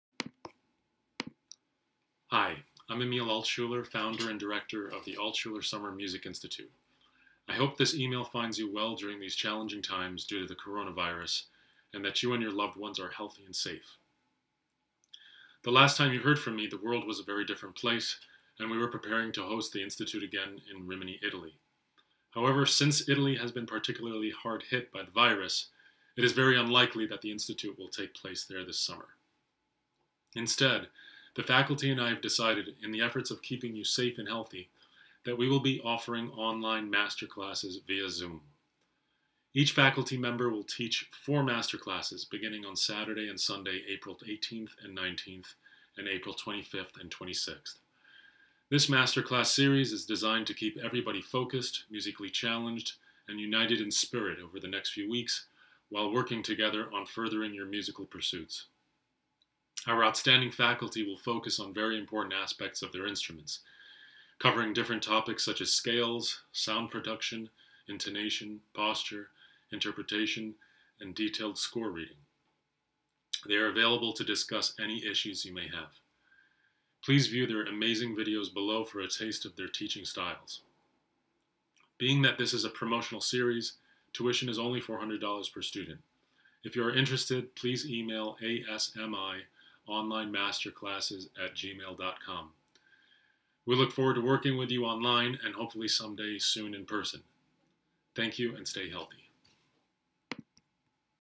Altschuler online intro
I hope you are all doing well during these challenging times living through the Covid-19 pandemic. As many of you have been wondering, it is very unlikely that we will be able to host ASMI in Rimini again this summer. However, for the sake of everybody's safety and well being, we will be offering online masterclasses via Zoom. Each faculty member will teach four masterclasses beginning on Saturday and Sunday, April 18/19 and April 25/26. This masterclass series is designed to keep everybody focused, musically challenged, and united in spirit over the next few weeks, while we working together on furthering your musical pursuits.

It is open to violinists, violists, cellists, bassists, guitarists, and pianists of all ages and levels. There is no application fee and the application deadline is April 15th. This is a promotional masterclass series, so the tuition is only $400 per student.
The faculty will include: Violinist/violist, Victor Romanul, cellist Michael Haber, guitar Prof. Piero Bonaguri from Bologna Conservatory, double bass/bass Prof. Claudio Campadello from Pesaro Conservatory, violinist/violist Prof. Joung Hoon Song from New England Conservatory, and Minkyung Oh from Rivers School Conservatory onto the faculty. We also welcome back pianist Marvin Wolfthal.

Please view their introductory videos below for details on their masterclasses and forward this opportunity anyone who may be interested in participating. Thank you, and we hope to work with you online toward your goals for this very exciting and unique opportunity!

Emil Altschuler
Director & Founder